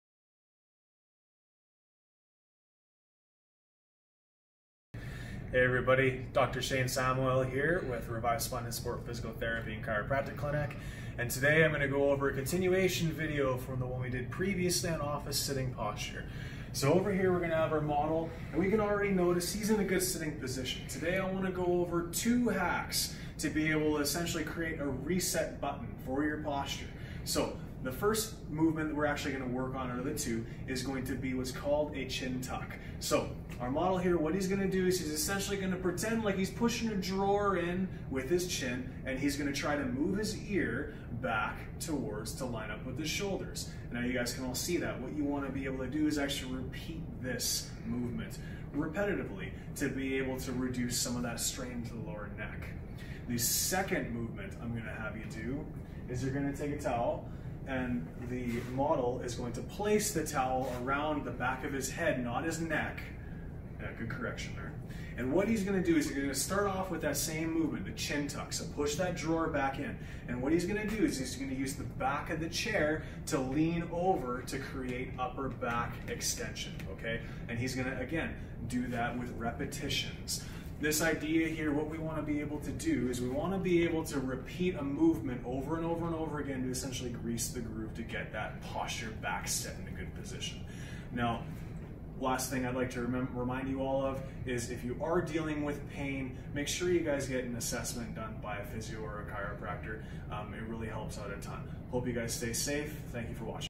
Couple of movement hacks to help and prevent neck pain and posture, key tips from a chiropractor.
Dr. Shayne Samoil talks about a few movement hacks that can help and prevent neck pain. If you have any questions message us.